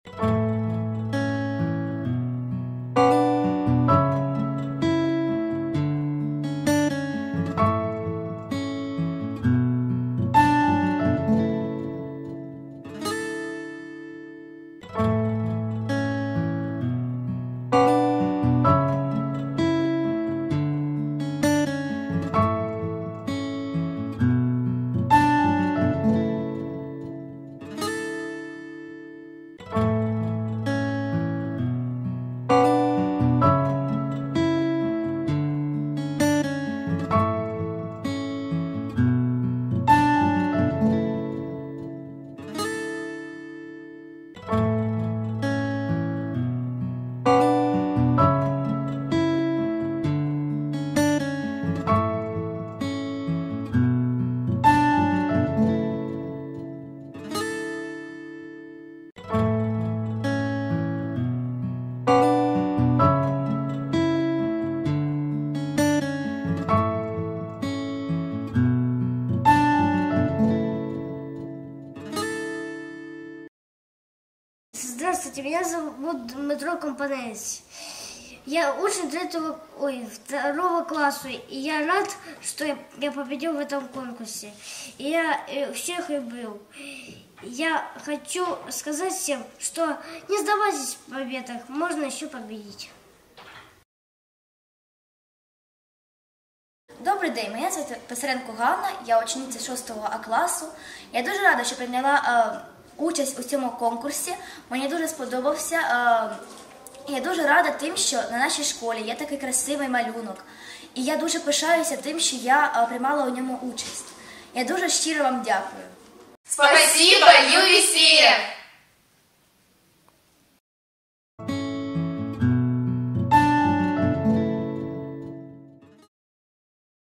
Відео-подяка фонду UWCF від школярів з Авдіївки
Відео-подяка фонду UWCF від учнів Авдіївської загальноосвітньої школи І-ІІ ступенів № 1 за проведені конкурс малюнків «SOLOwayART»  на тему «Пісні солов’я» та Історико-мистецького дослідження на тему «Українська та японська культура: так далеко і так близько», відзнаки переможців та подарунки всім учасникам.
Нагадаємо, що зазначені конкурси були проведені Благодійним фондом "Об'єднання світових культур" в рамках реалізації Соціально-художнього проєкту «SOLOwayART», в результаті конкурсу соціальних проєктів «Перетворимо Авдіївку разом» від ГО «Авдіївка. Платформа спільних дій» та Військово-цивільної адміністрації м. Авдіївка за підтримки компанії «Метінвест».